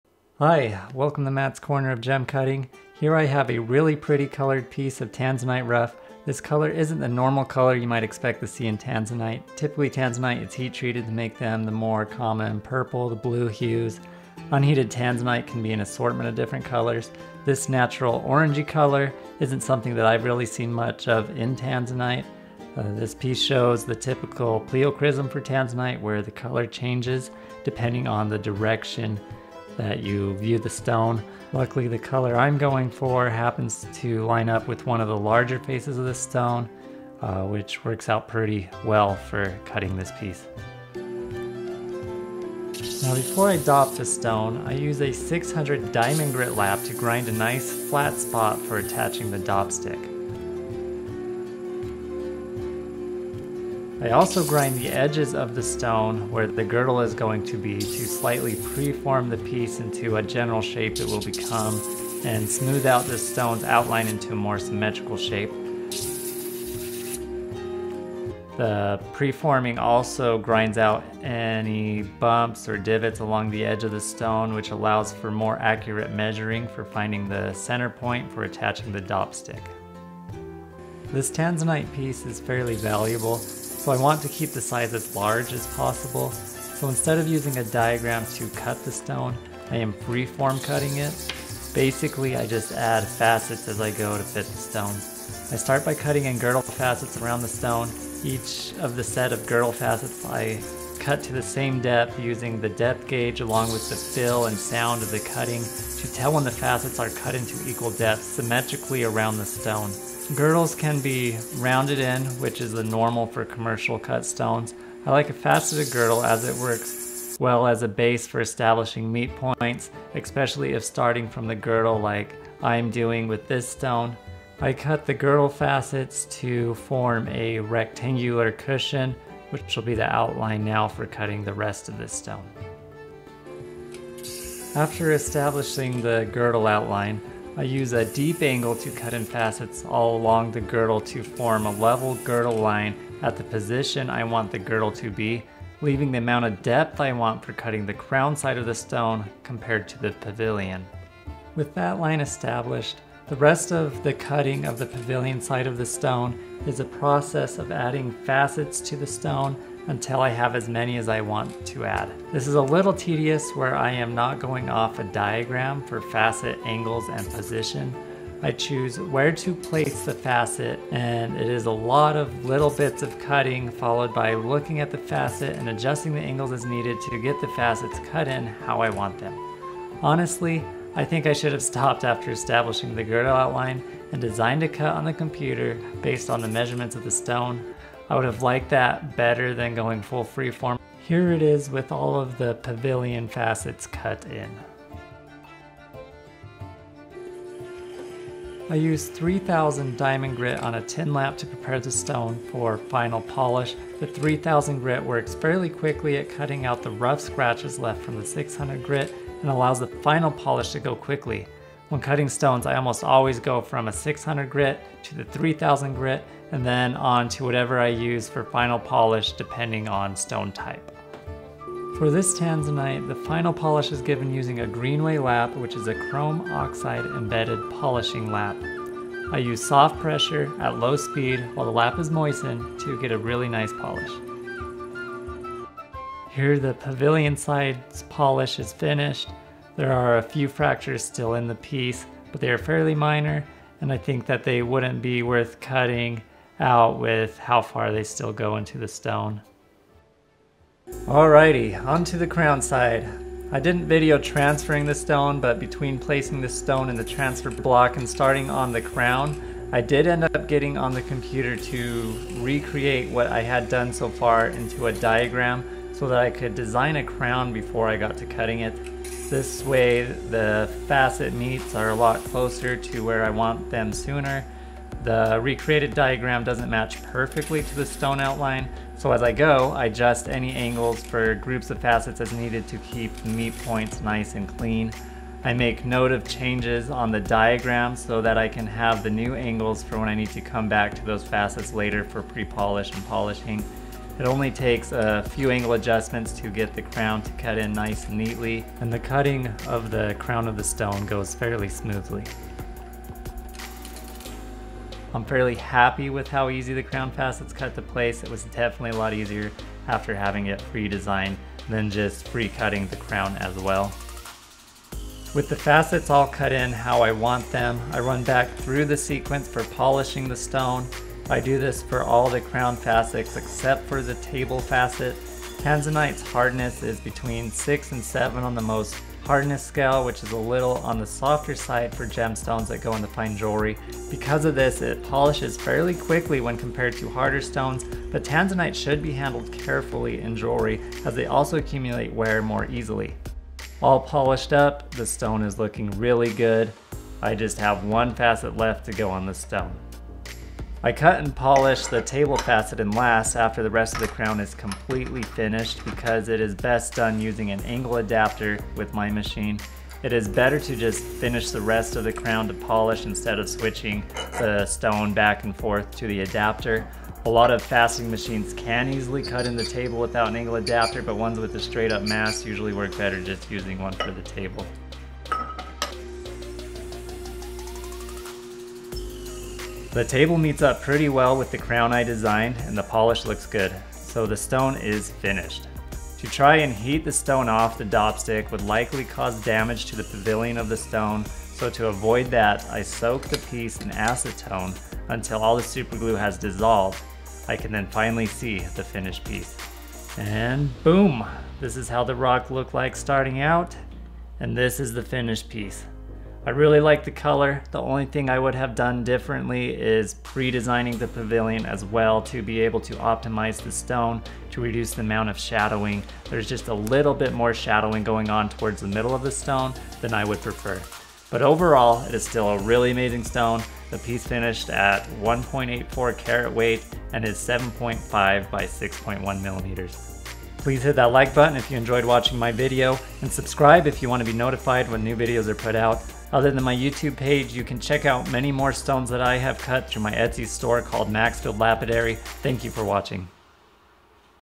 Cutting a Natural Colored Tanzanite, Bringing the Gem out of the Rough
This is the natural color of this untreated tanzanite. I go through how I facet this stone starting with free-form cutting and creating the cut as I go to fit the stone to save the stones size.

40x Loupe
Digital Caliper
Gorilla Super Glue Gel
Liquid Super Glue
6 Piece Tweezers Set

Camera used in this video
Marco attachment used for close ups